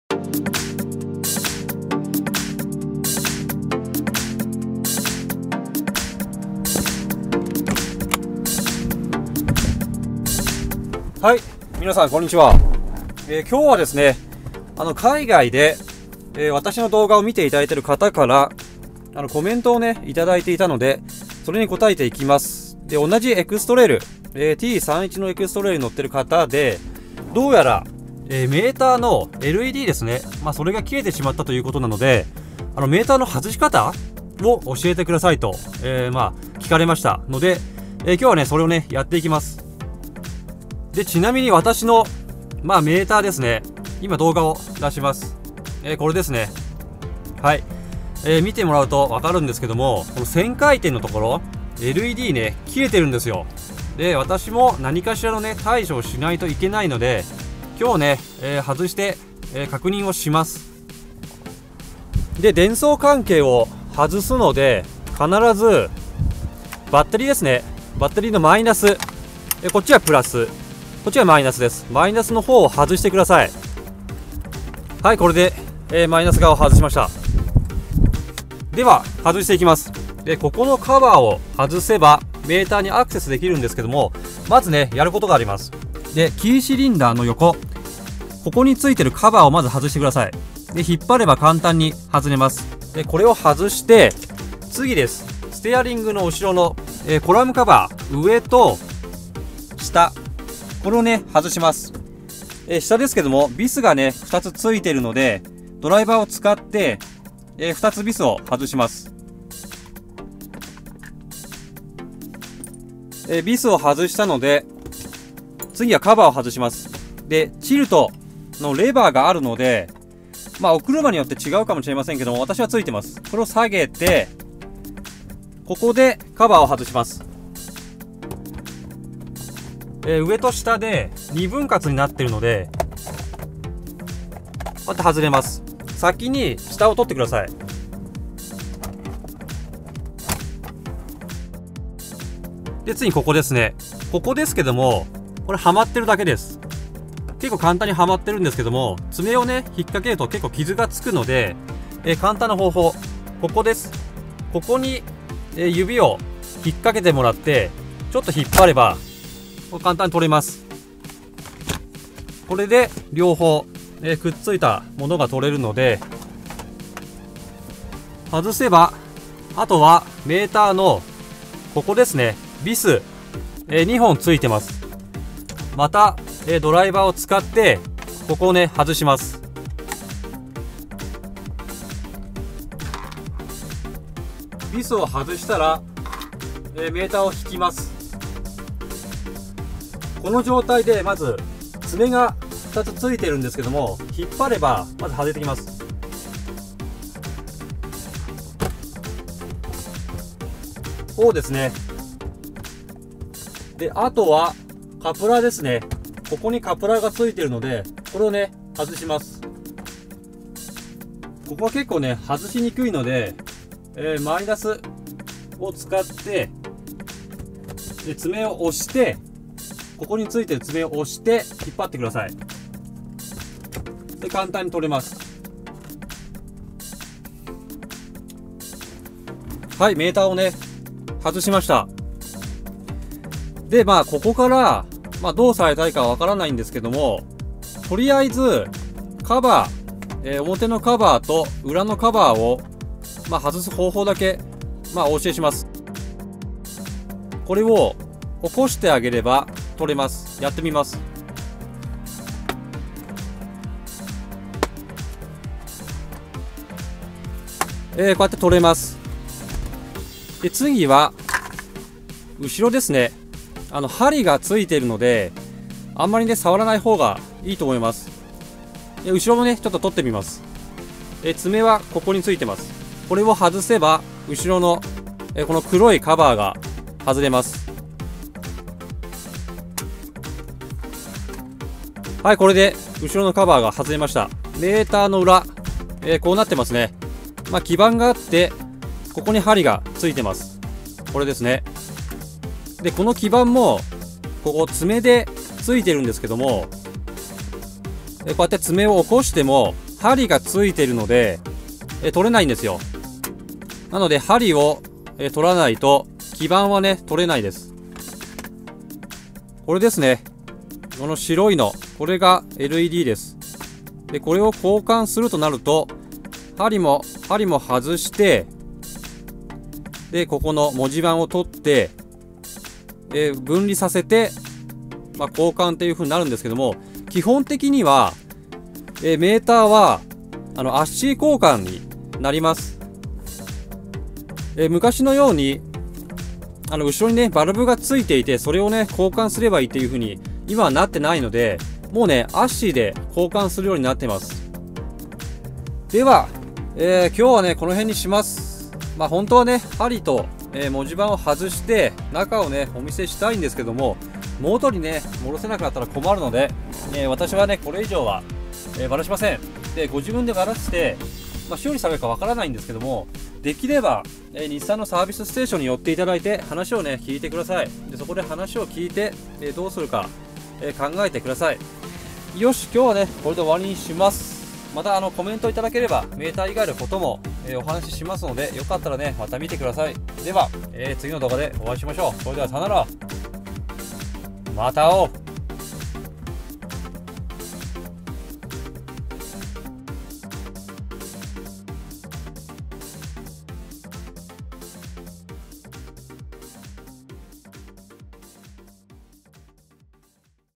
X-TRAIL T31 / How to remove the meter /【ｺﾒﾝﾄ回答】ﾒｰﾀｰの外し方
I received your comment on how to remove the meter so I will answer.
メーターの外し方のコメントをいただいたのでお答えいたします。